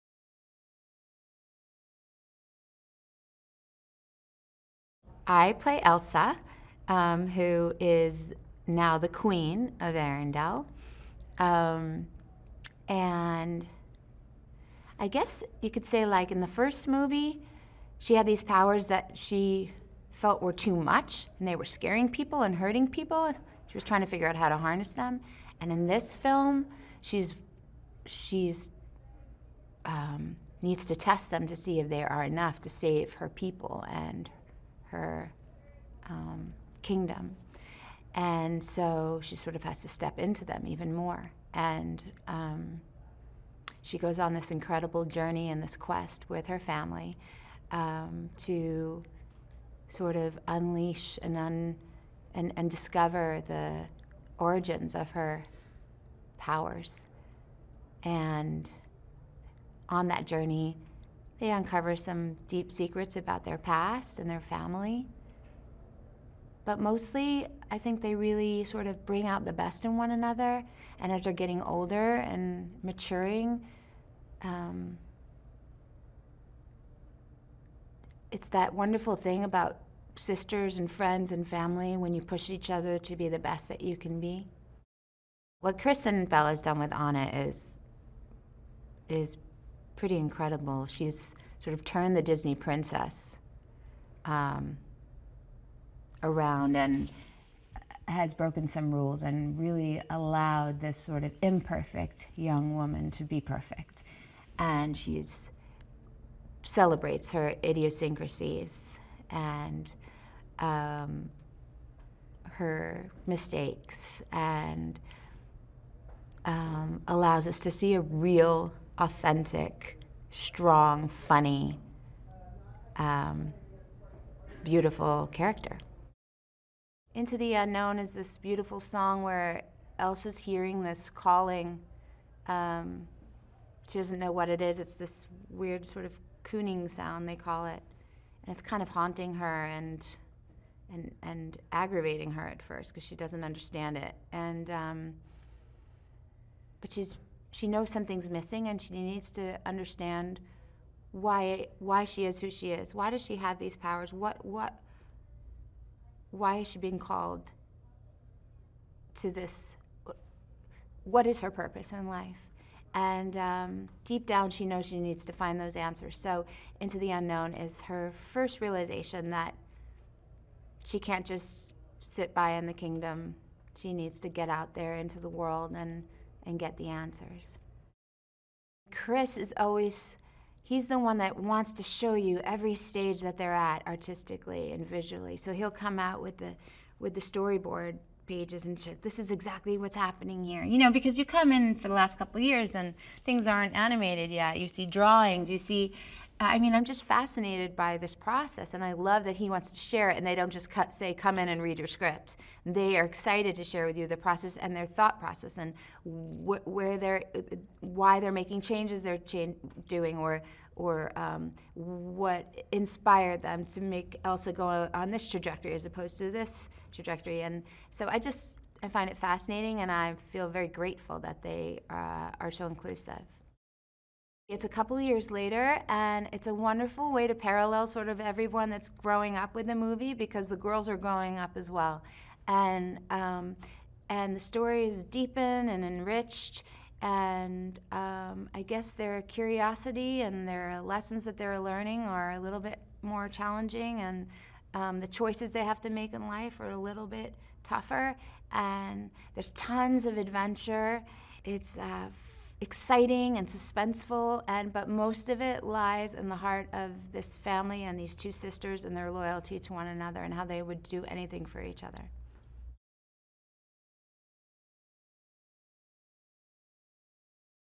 Frozen 2: Idina Menzel "Elsa" Behind the Scenes Movie Interview | ScreenSlam
Frozen 2: Idina Menzel "Elsa" Behind the Scenes Movie Interview